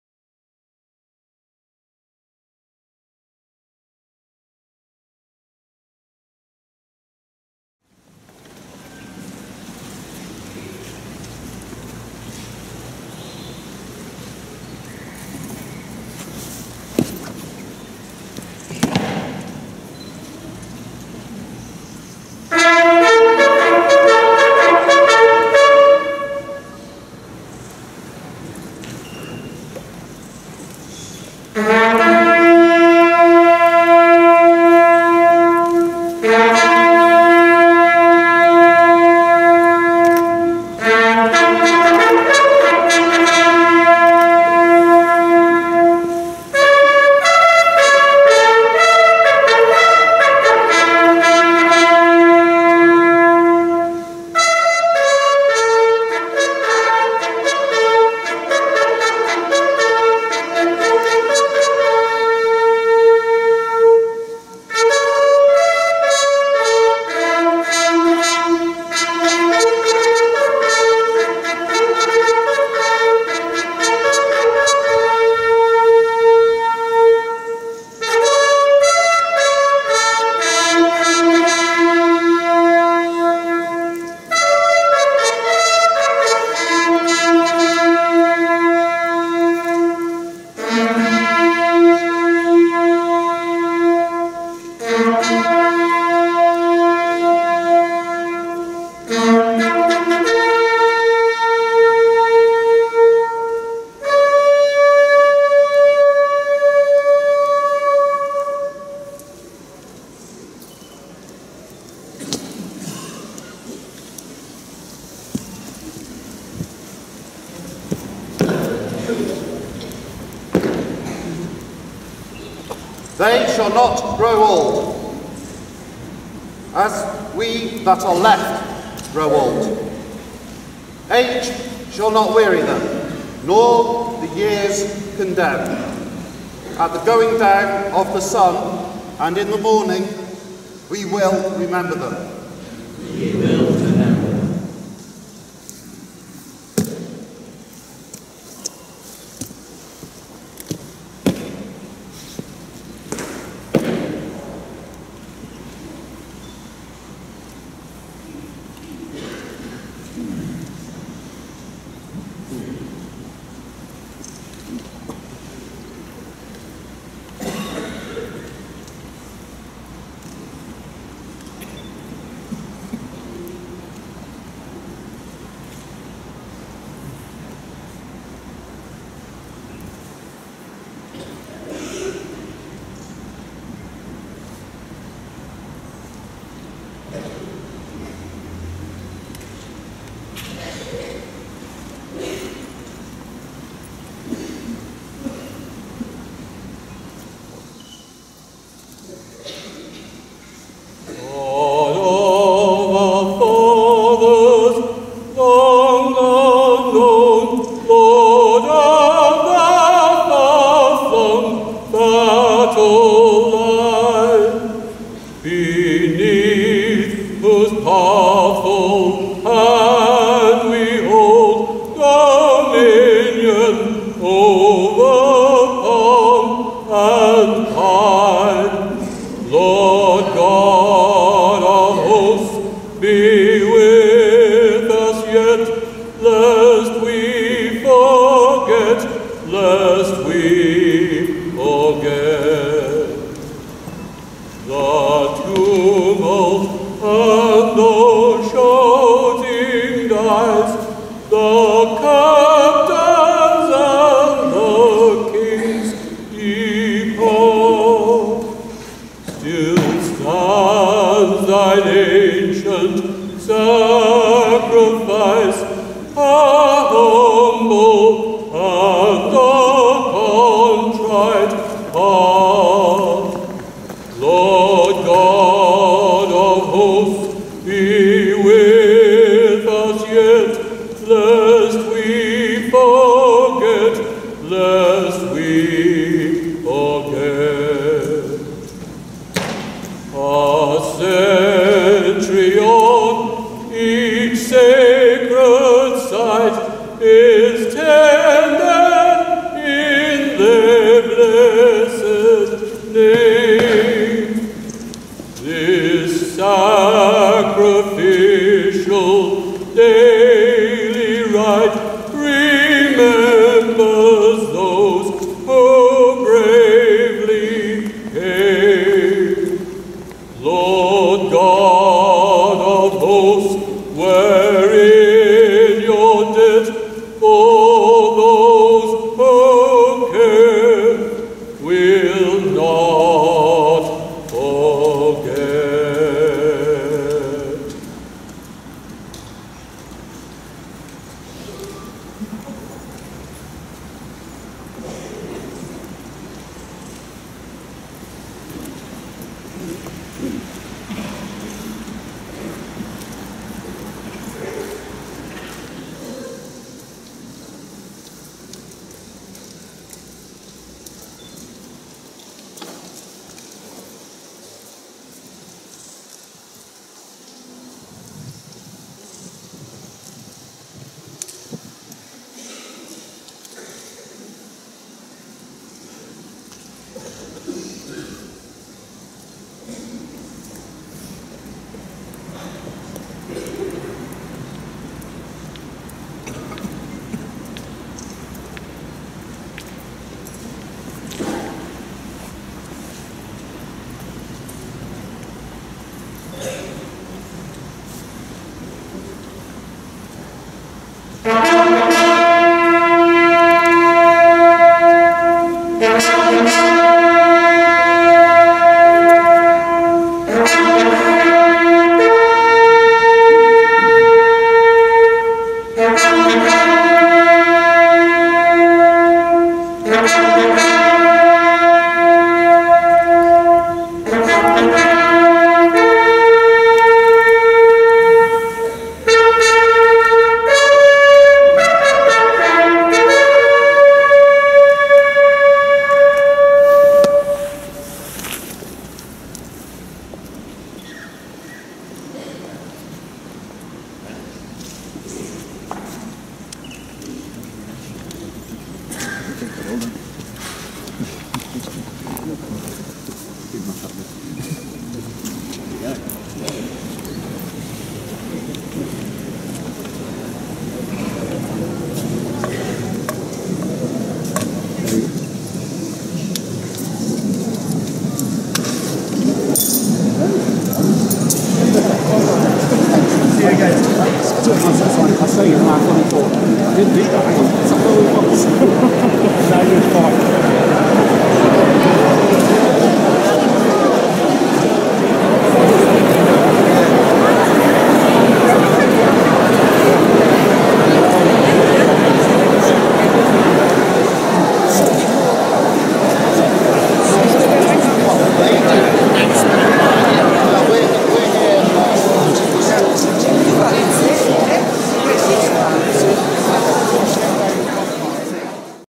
Neville Biggs singing at Menin Gate, Ypres
Neville singing at the Last Post Ceremony at Menin Gate, Ypres, Belgium.